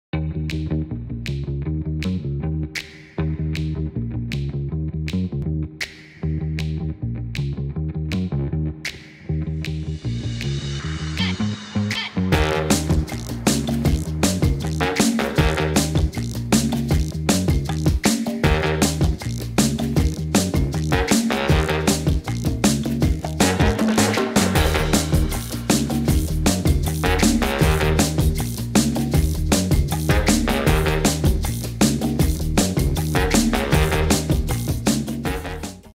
Recipe Substitutions Every Home Baker Should Know
Have you ever gotten halfway through a baking recipe only to realize you’re missing a crucial ingredient? With these common baking ingredient substitutions, you can avoid another trip to the store. We'll share substitutes for butter, substitutes for buttermilk, substitutes for baking powder, and more. Trust us, you’re going to want to bookmark this one!

Substitute for Buttermilk: 00:02
Substitute for Half and Half: 00:04
4 Substitutes for Butter: 00:07
Substitute for Baking Powder: 00:16
Substitute for Baking Soda: 00:19
4 Egg Substitutes for Baking: 00:22
Substitute for Corn Syrup: 00:28